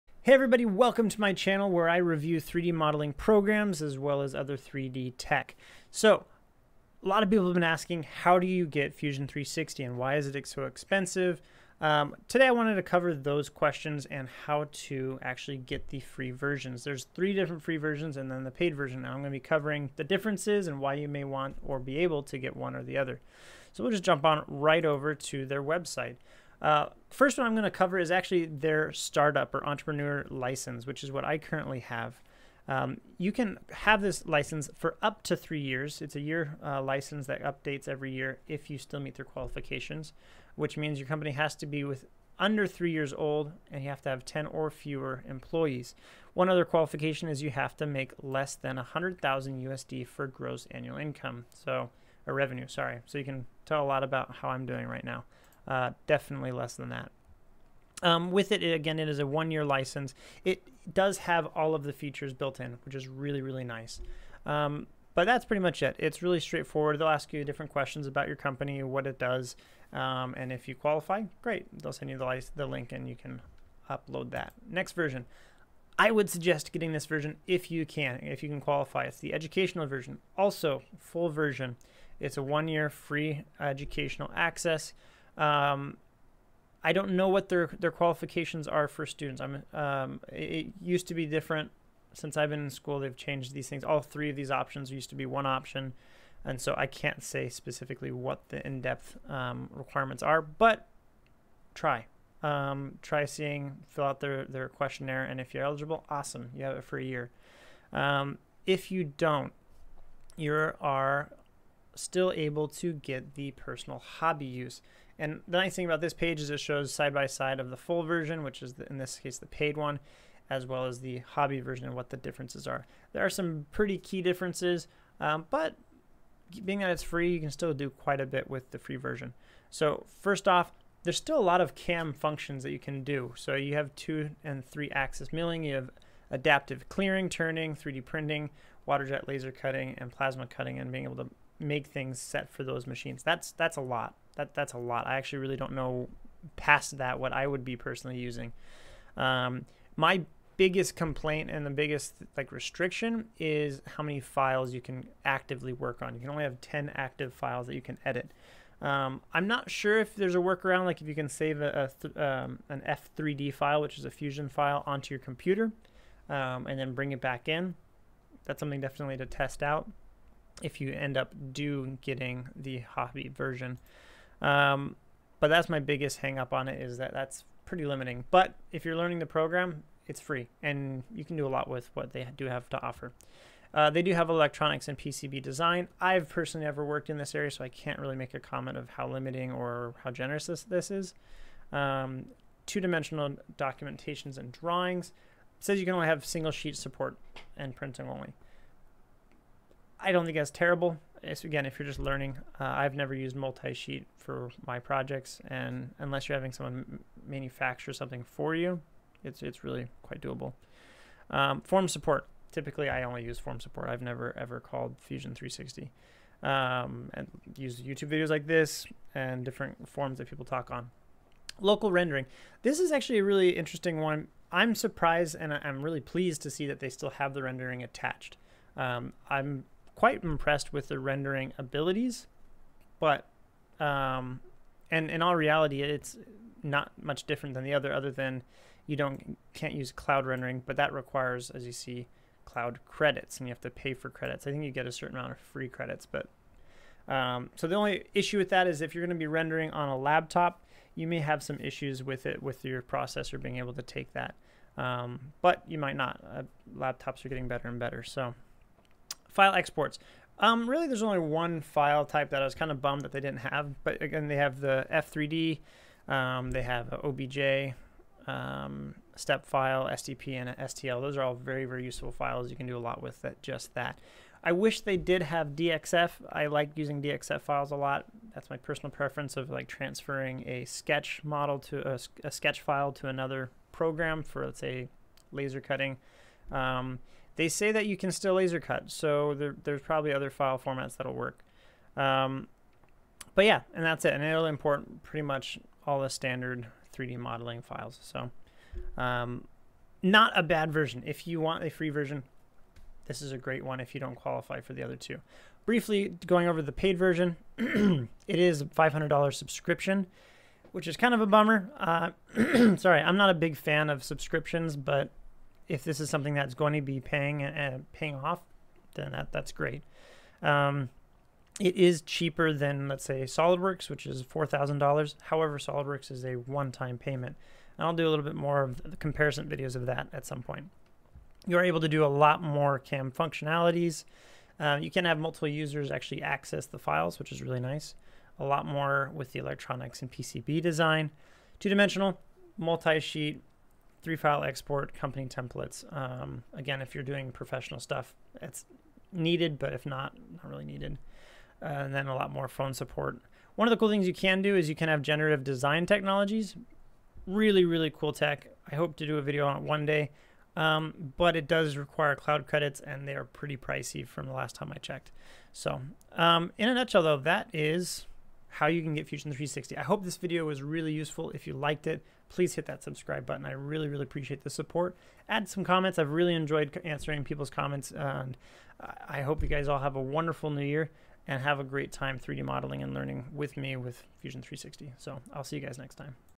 How to get Fusion 360 FREE
Sounds like click bait, but that is what this video is about. Multiple people have been asking how to get Fusion 360 so I wanted to make a tutorial on just that. I have attached the AutoDesk links below for the various options.

Start-Up Fusion 360 License (Free for up to 3 years *with conditions)